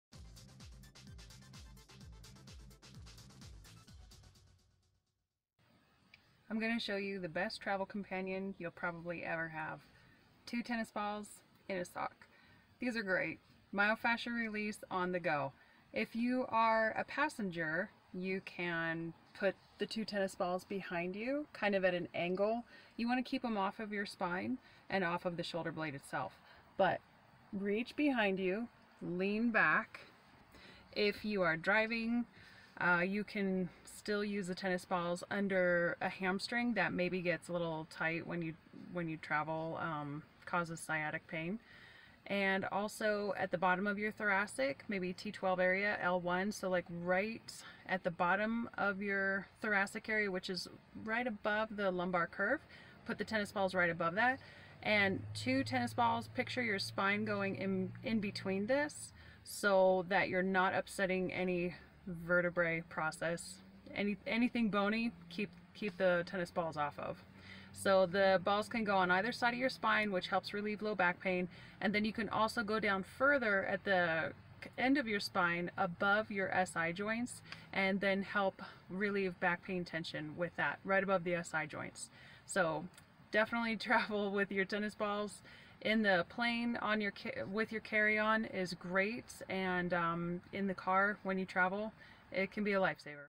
Back Pain while traveling: solution: tennis balls
We all get aches traveling- in cars, in planes, even in hotel beds! this lightweight, small, and low-cost DIY device can really help get you out of a tight spot!  ;)

StraightLineFS.com